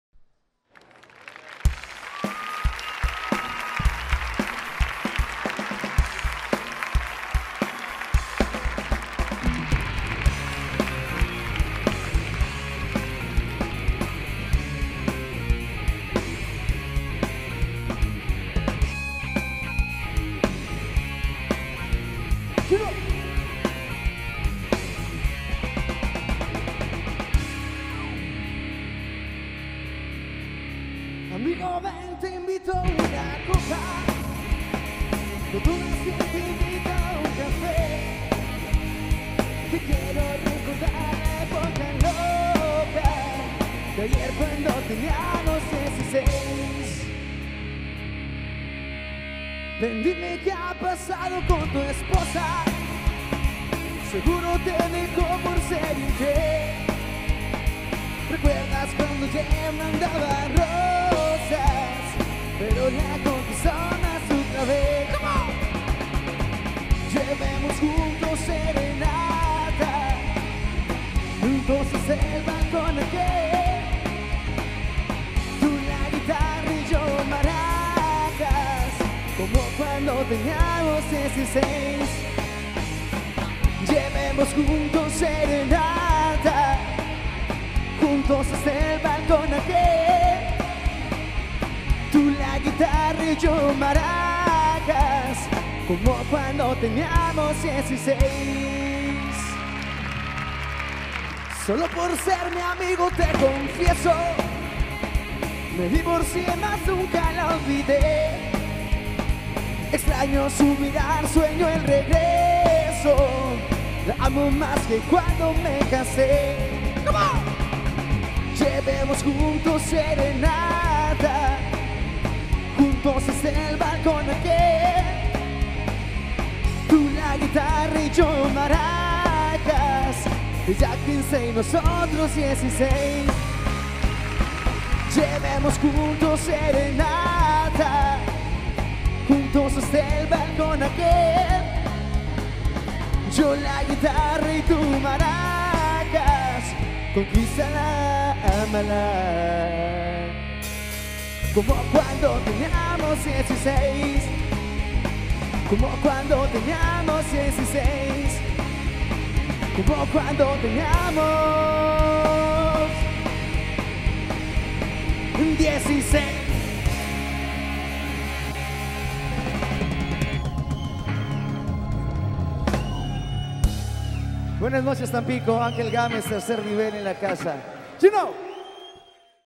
Angel Gámez / Maracas
Este video fue tomado en las Fiestas de Abril de este año, esperando que les guste y sea de su agrado.
Felicidades Angel Gámez y Tercer Nivel.
Información en Facebook
/umediatv
/angelgamez